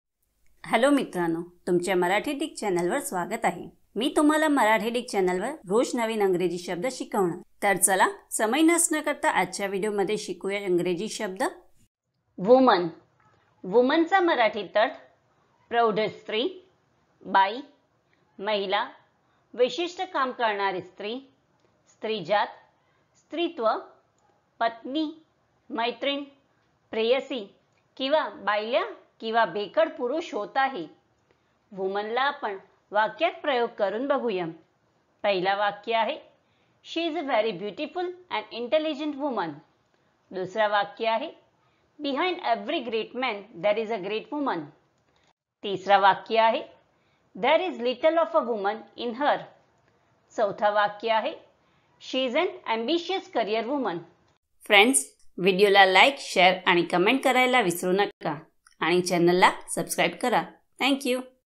Woman Meaning in Marathi | Woman म्हणजे काय | Woman in Marathi Dictionary
या व्हिडिओमध्ये तुम्हाला मराठीत Woman चा अर्थ समजेल आणि Woman चा उच्चारही शिकाल. याचा अर्थ तुम्ही Woman चा अर्थ जाणून घ्याल तसेच Woman कसे उच्चारता येईल, Woman म्हणण्याचा योग्य मार्ग कोणता आहे.

तर Woman शब्द फक्त एका मिनिटात शिका.

उदाहरणांसह Woman समजून घेऊ.